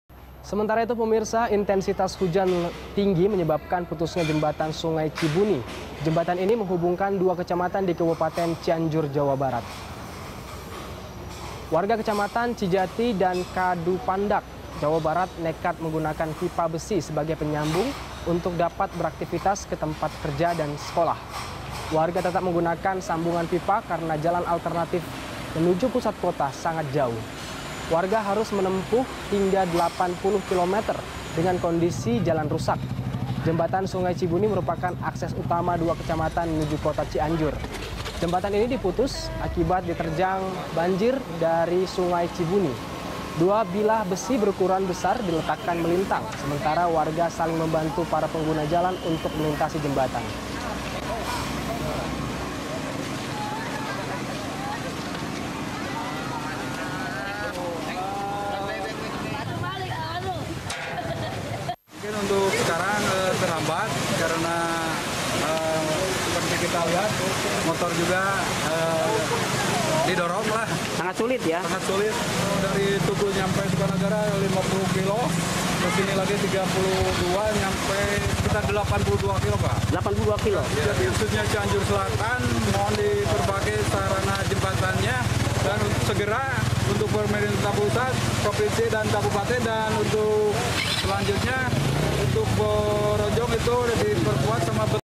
Jembatan Penghubung Dua Kecamatan di Cianjur Putus
Intensitas hujan tinggi menyebabkan putusnya jembatan Sungai Cibuni. Jembatan ini menghubungkan dua kecamatan di Kabupaten Cianjur, Jawa Barat.

Facebook.com/BeritaSatuTV
Youtube.com/BeritaSatu